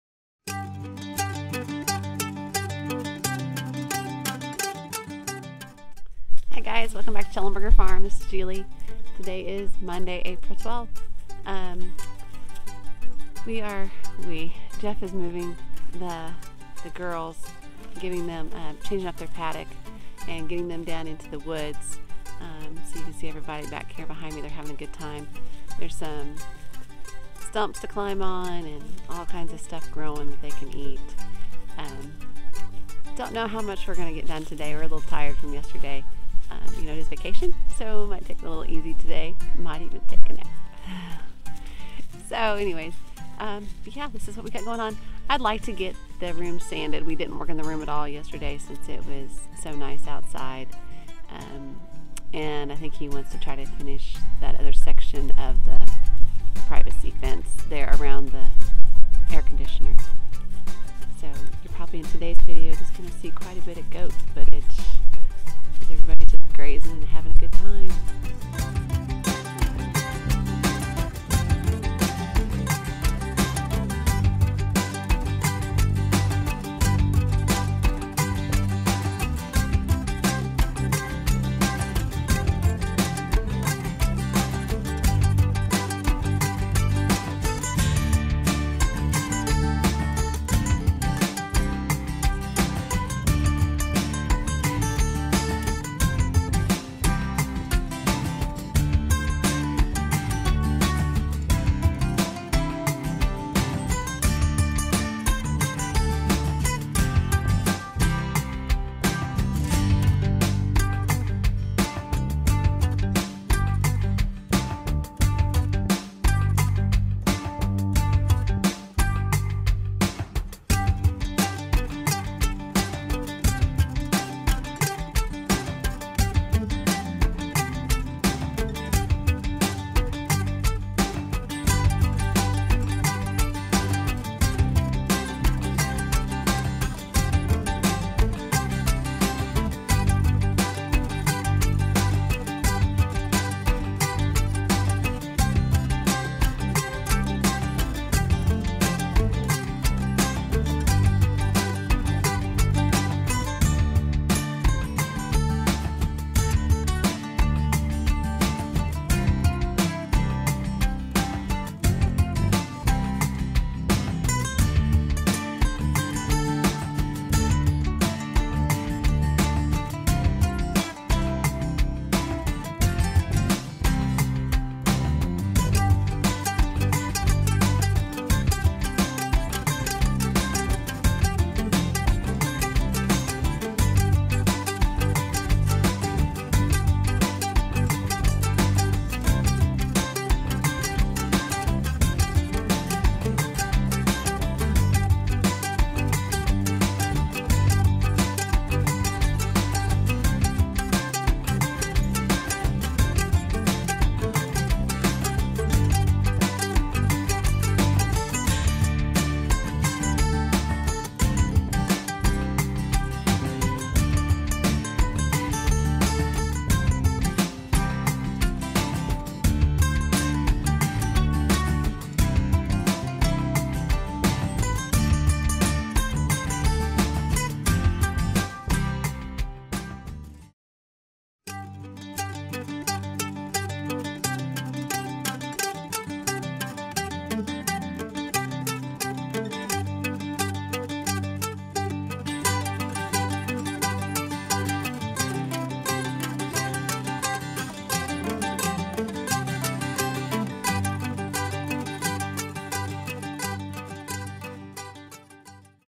Vacation Vlog 04.12.21
We got more done today than we thought we would.  I really enjoyed being outside with the goats.